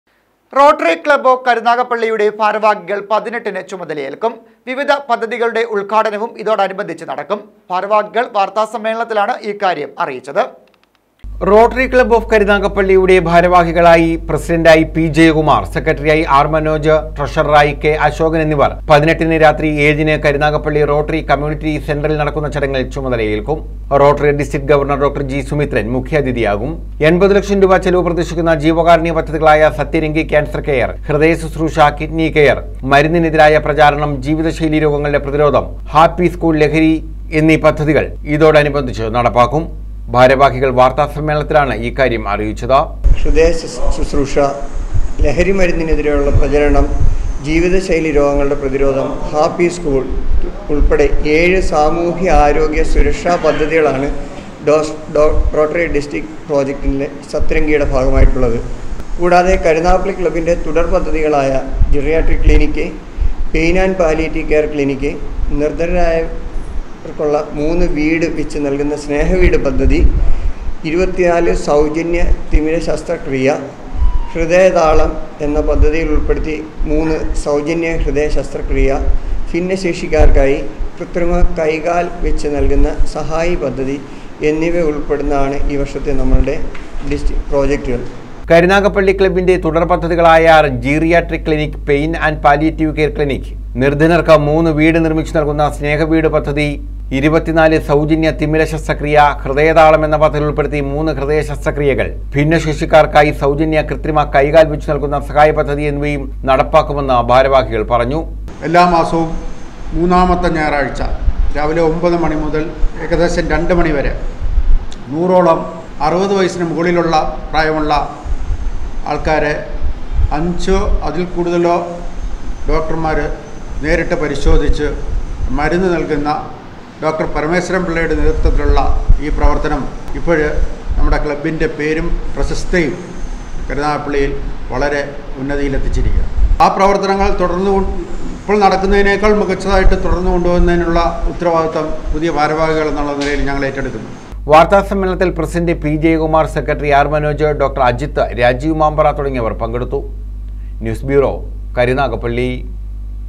റോട്ടറി ക്ലബ്ബ് ഓഫ് കരുനാഗപ്പള്ളിയുടെ ഭാരവാഹികള്‍ 18 ന് ചുമതലയേല്‍ക്കും
റോട്ടറി ക്ലബ്ബ് ഓഫ് കരുനാഗപ്പള്ളിയുടെ ഭാരവാഹികള്‍ 18 ന് ചുമതലയേല്‍ക്കും.വിവിധ പദ്ധതികളുടെ ഉദ്ഘാടനവും ഇതോടനുബന്ധിച്ച് നടക്കും. ഭാരവാഹികള്‍ വാര്‍ത്താസമ്മേളനത്തിലാണ് ഇക്കാര്യം അറിയിച്ചത്.